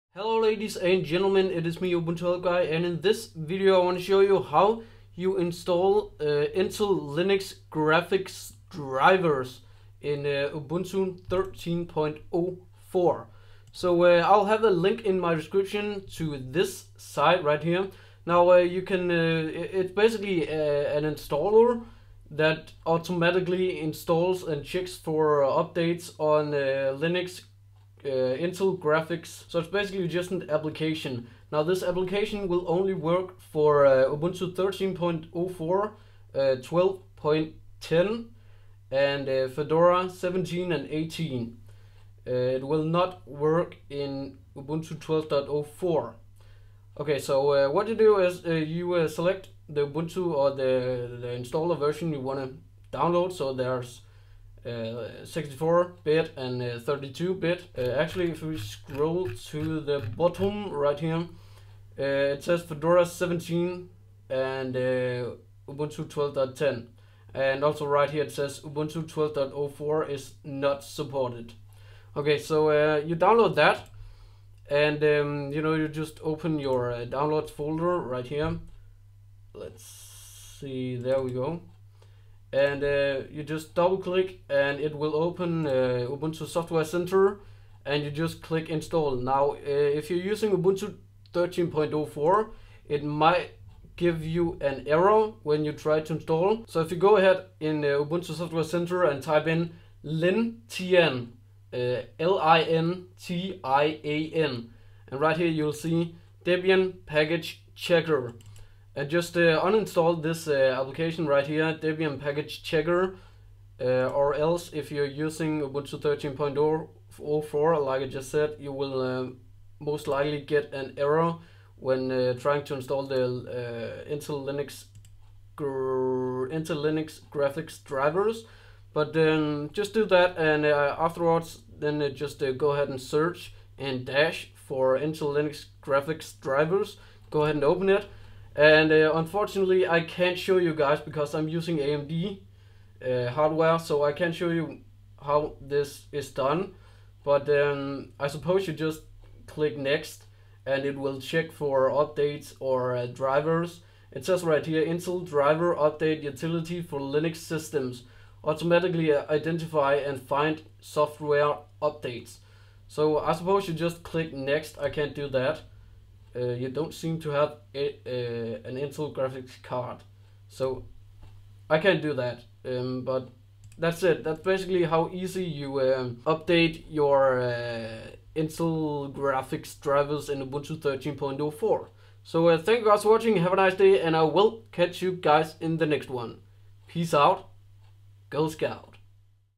Linux Intel Graphics Drivers - Ubuntu 13.04
In this video, I show you an application that lets you stay up to date with the latest Intel Graphics drivers in Ubuntu. This app works for Ubuntu 13.04 and 12.10 - It also works with Fedora 17 and 18. Remember to like the video and also leave a comment on your thoughts!